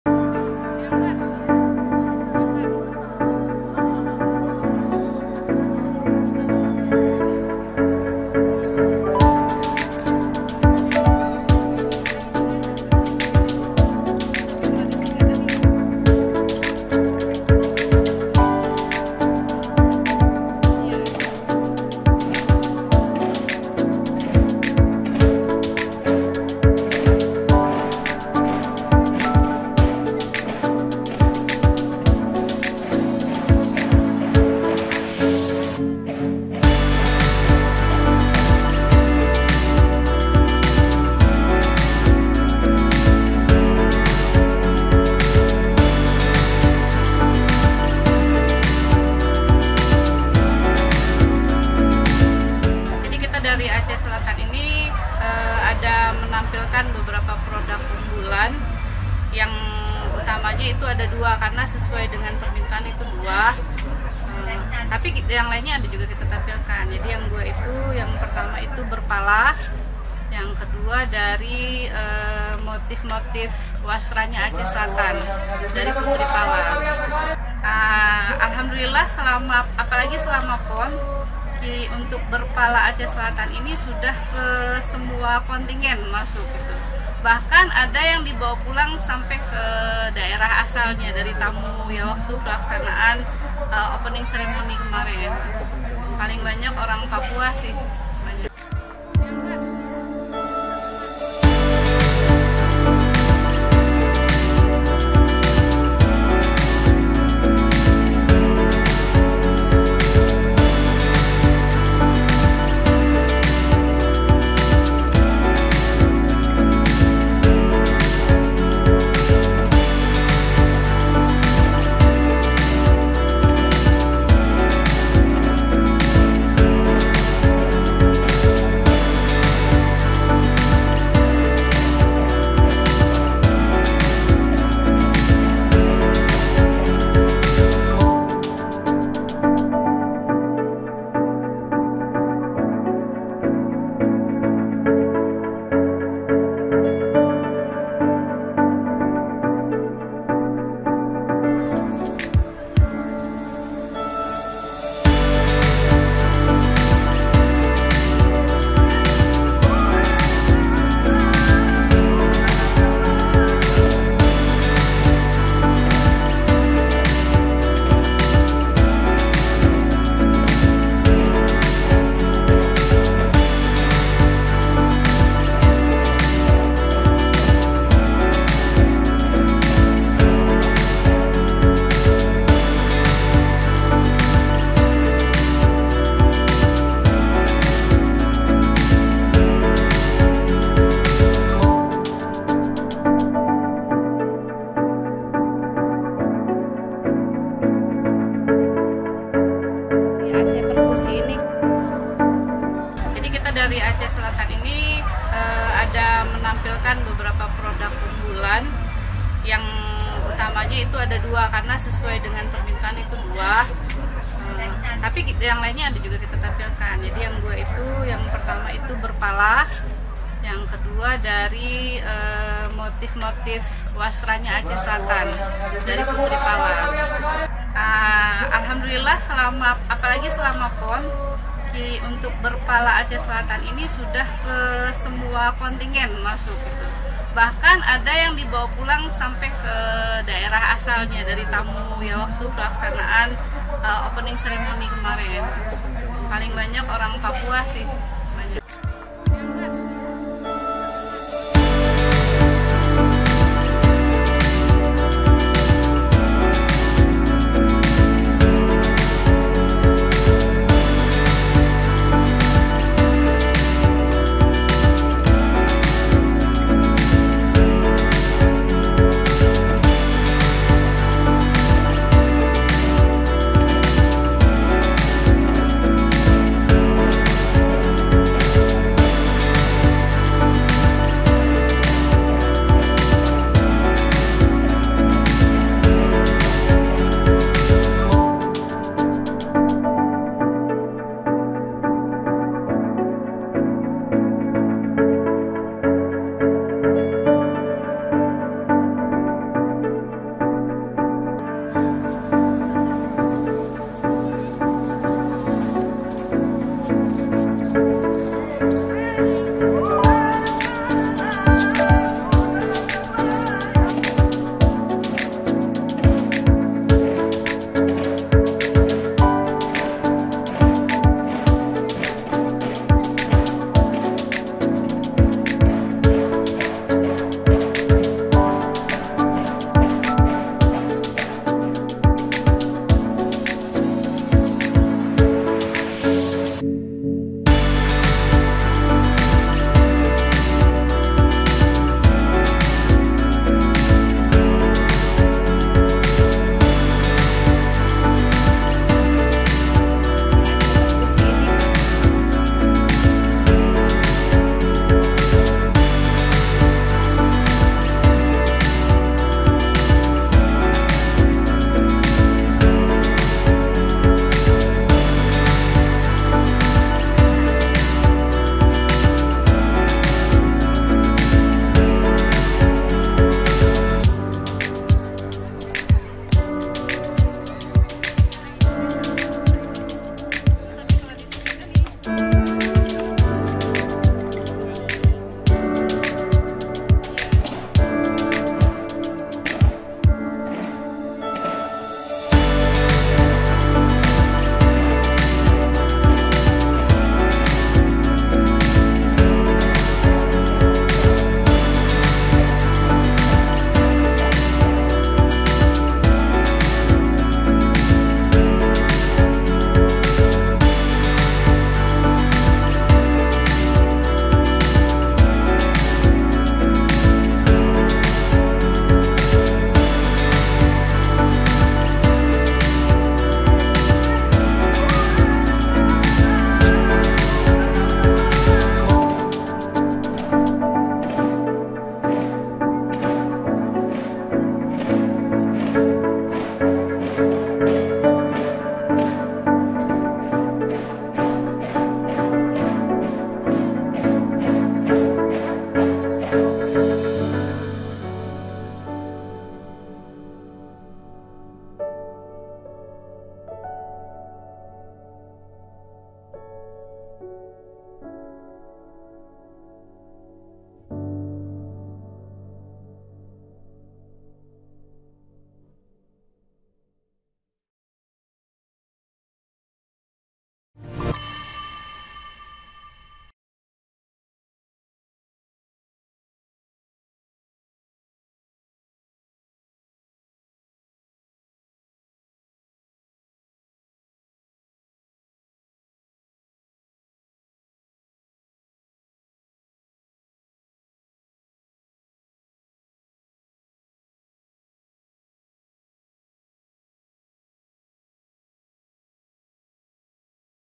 Kontingen PON Aceh-Sumut Serbu Produk Lokal: Tenun dan Minuman Pala Jadi Favorit
Pekan Olahraga Nasional (PON) XXI Aceh-Sumut tidak hanya mempertandingkan berbagai cabang olahraga, tetapi juga menjadi ajang untuk mempromosikan kuliner dan oleh-oleh khas dari berbagai daerah, salah satunya Aceh Selatan.

Salah satu acara yang memeriahkan PON Aceh adalah pagelaran Perkusi Expo, yang menghadirkan UMKM dari 23 kabupaten/kota di Aceh untuk menampilkan produk unggulan mereka.

Minuman berbahan dasar buah pala atau dikenal sebagai "Berpala" serta tenunan khas Aceh Selatan yang dijajakan di stand UMKM rupanya jadi primadona dan menjadi incaran kontingen dari luar Aceh.

Perkusi Expo sendiri berlangsung dari 13-18 September, diikuti oleh ratusan pelaku UMKM dari seluruh Aceh.